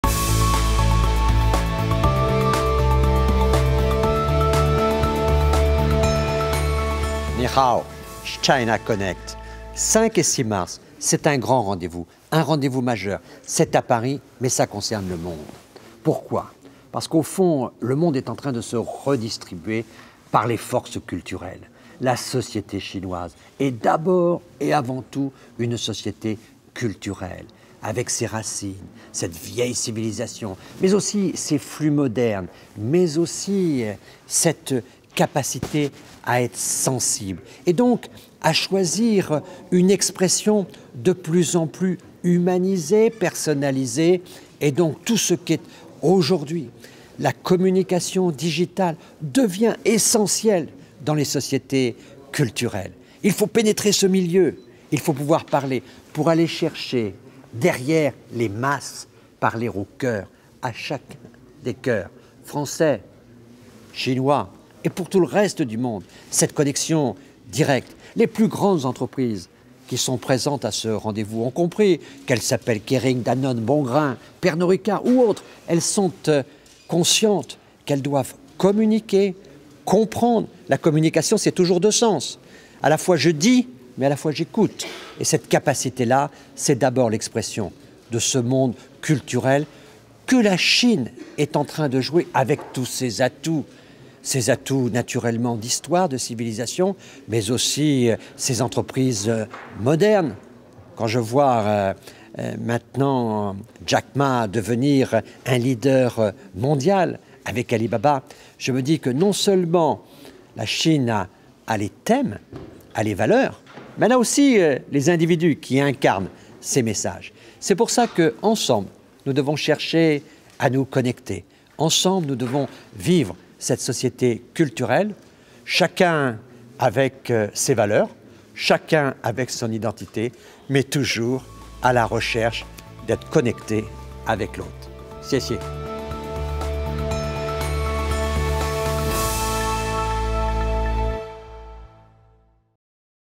Jean Pierre Raffarin, Former French Prime Minister, Supports China Connect (Eng Subs)
Shot on February 12, 2015 at Le Sénat.

Direction: Kevin Lecomte
Production: Digiprod
Copyright: China Connect

5th Edition will take place on March 5-6, 2015 in Paris

Share this on Twitter

Jean-Pierre RAFFARIN : Ancien Premier Ministre | Président de la Commission
des Affaires Etrangères, de la Défense et des Forces Armées du Sénat